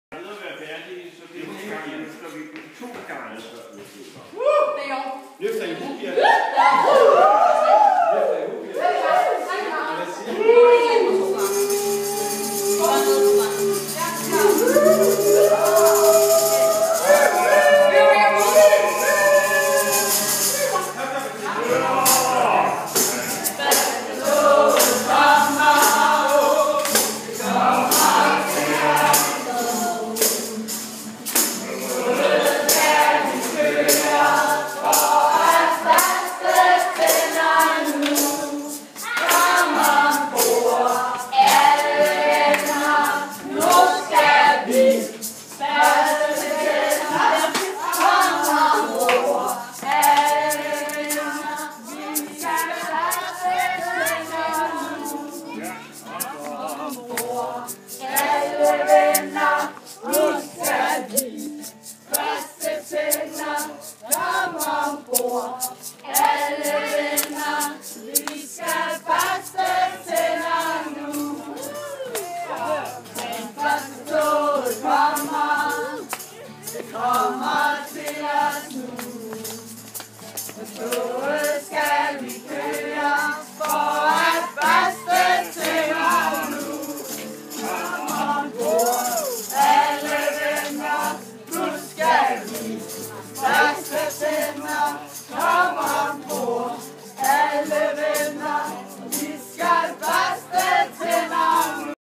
Tandbørstetoget 2014, Solborgen
MSA, Solborgen. D.4/7-14
Tandbørstetoget, det sidste af slagsen på denne sommers lejr.